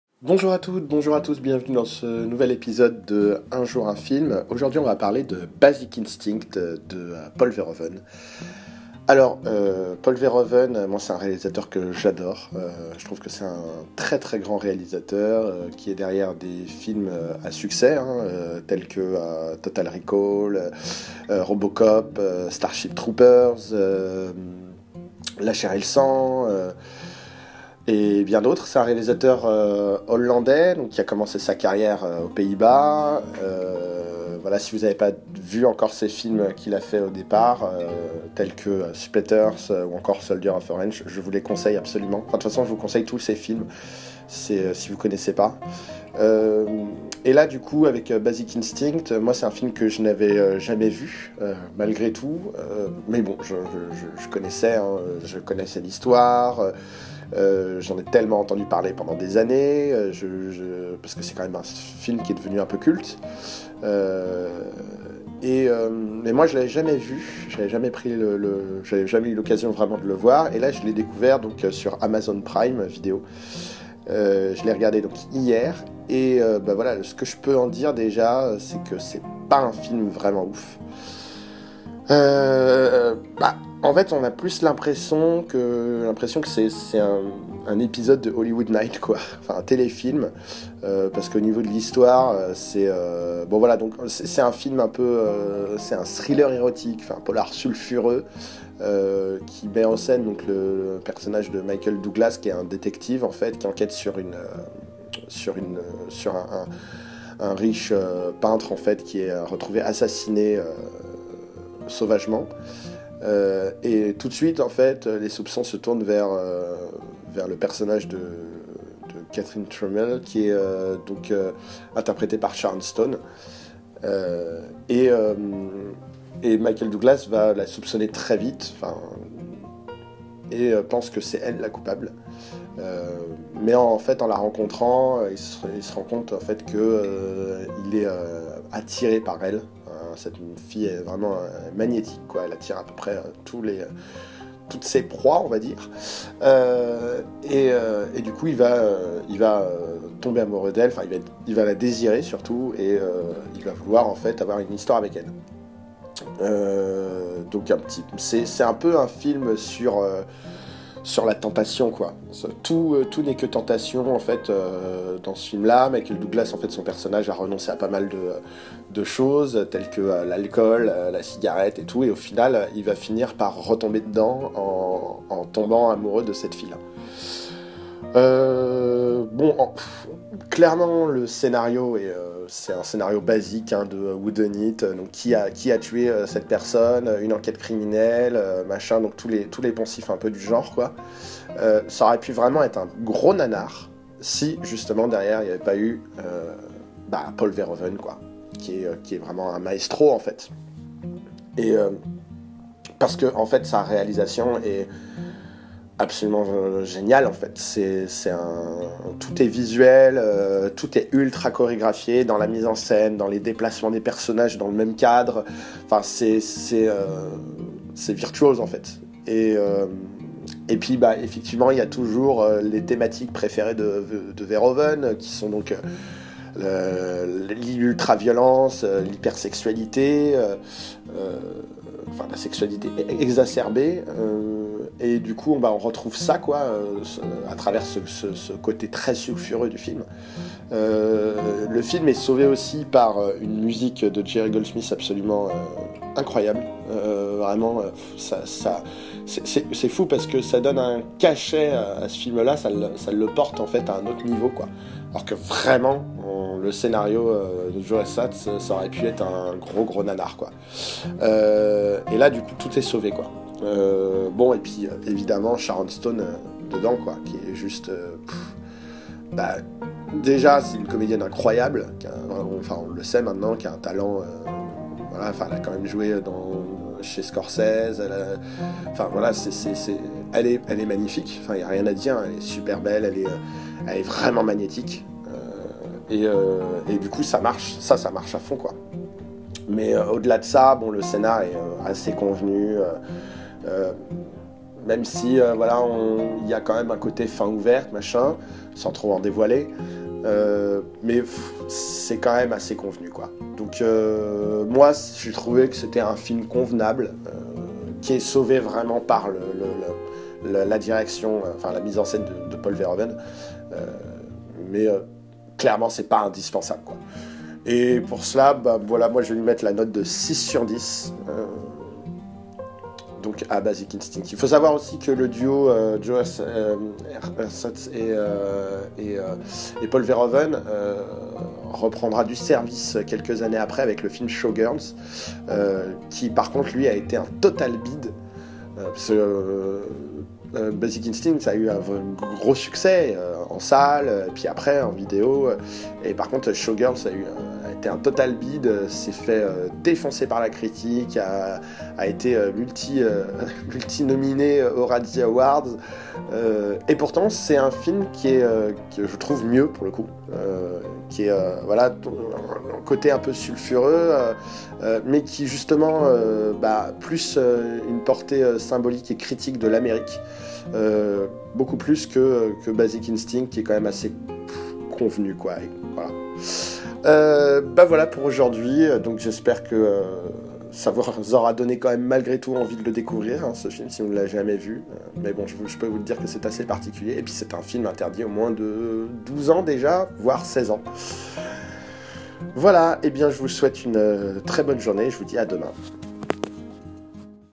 UN JOUR, UN FILM - BASIC INSTINCT #4
Téléfilm ou vrai film? Le grand Polo délivre un polar sulfureux qui est devenu culte. Un film de culte en soi. Bon c'est pas si étonnant que ça et surtout sauvé par sa mise en scène, sa musique et son actrice principale.
Note : 6/10
Retrouvez-moi sur